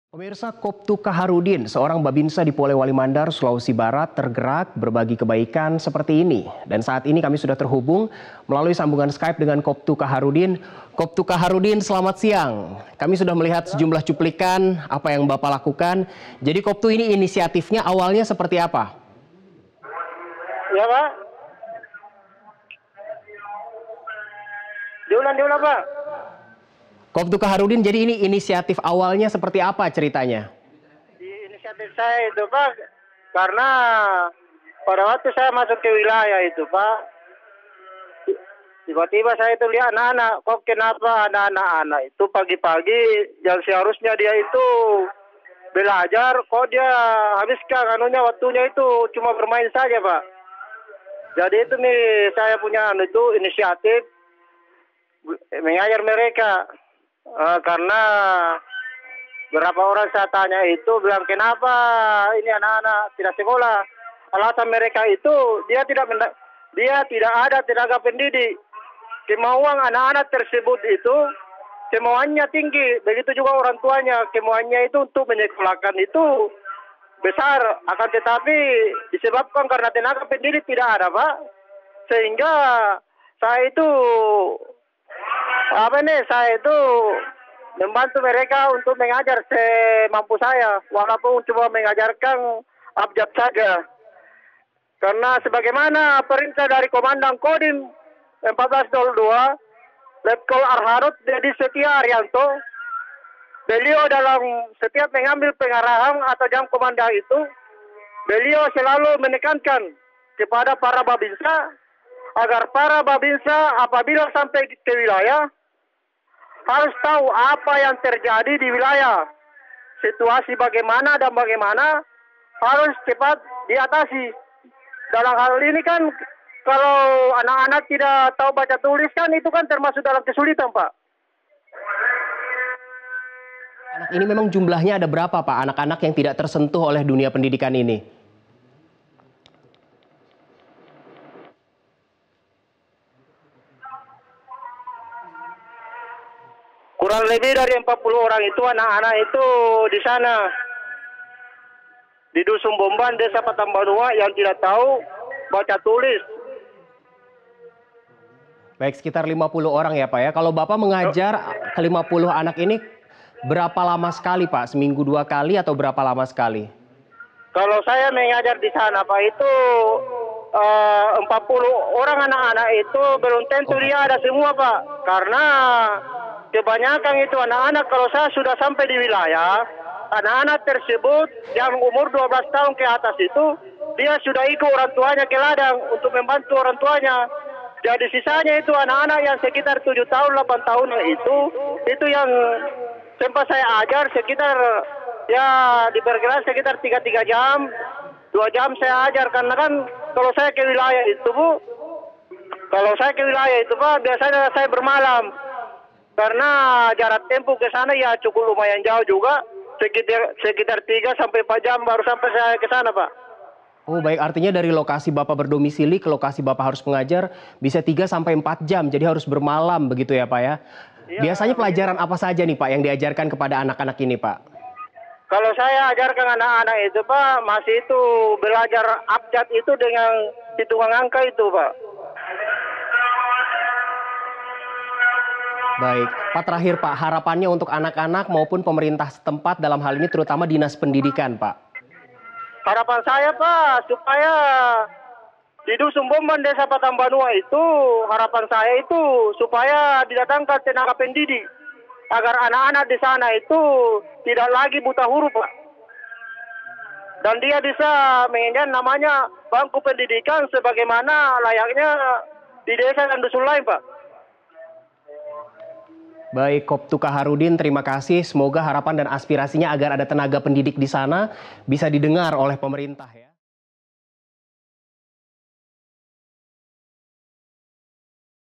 Cerita Babinsa Mengajar Anak Putus Sekolah di Pelosok
Koptu Kaharuddin, seorang Babinsa di Polewali Mandar, Sulawesi Barat tergerak berbagi kebaikan. Di luar tugas pokoknya, ia mengajar anak-anak putus sekolah di wilayah pegunungan.